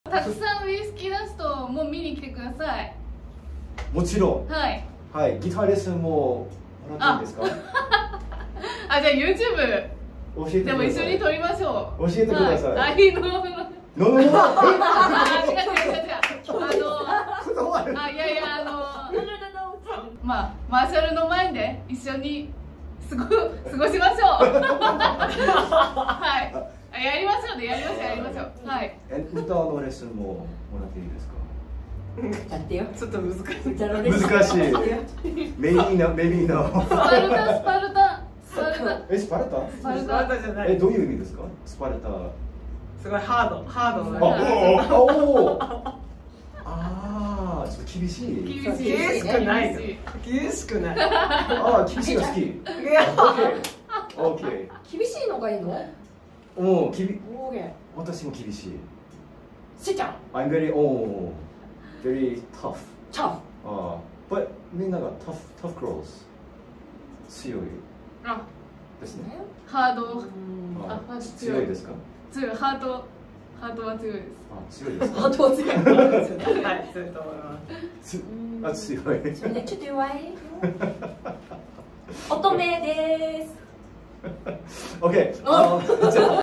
WHISKEY DUST x SALA INTERVIEW - COMING SOON!
An interview, four months in the making! Actually, four months in the waiting. Due to moving, work and just a lotta lot, my joint interview with Whiskey Dust and Sala has been on hold. But it's right around the corner! And it's a delight! Hang tight, folks!